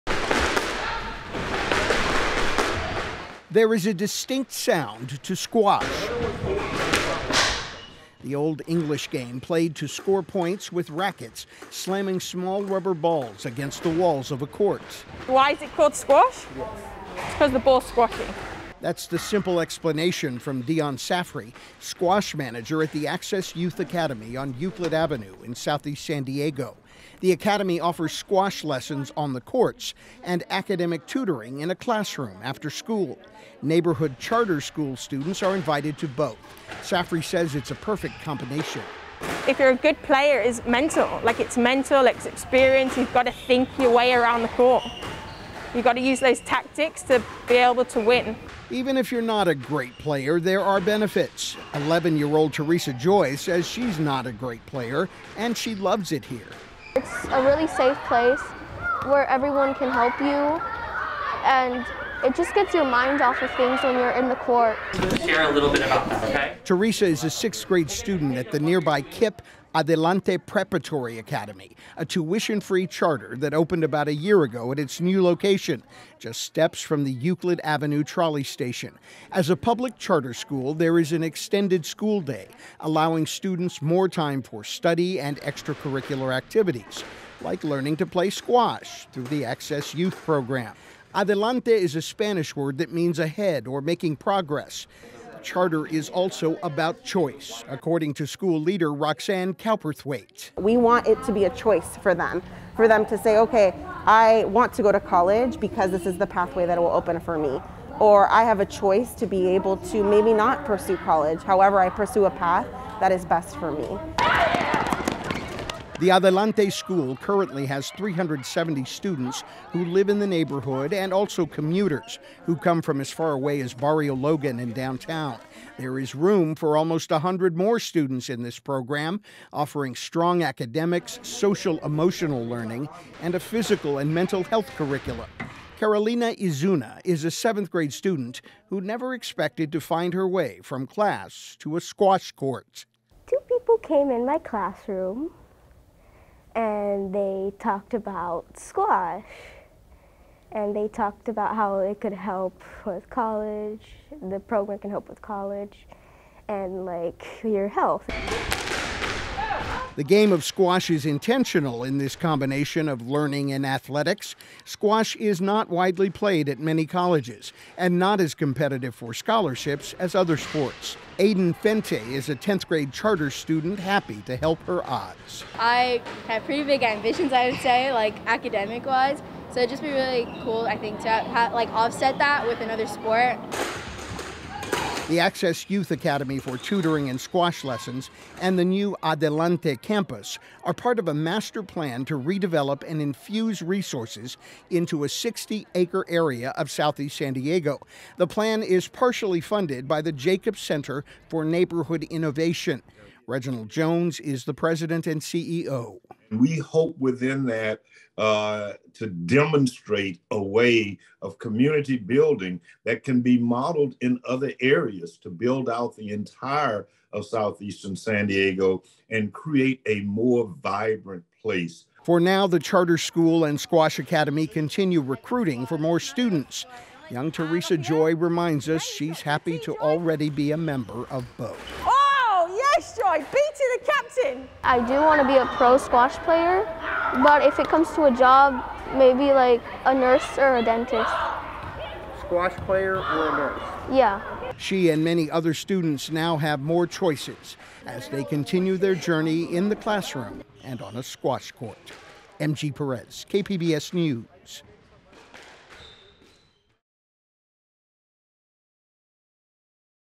Academics and squash give charter students a new choice for college
The community of Southeast San Diego is being infused with new resources to help children reach college and their career goals. KPBS Education Reporter M.G. Perez tells us about the unexpected collaboration between a charter school and the old English game of squash.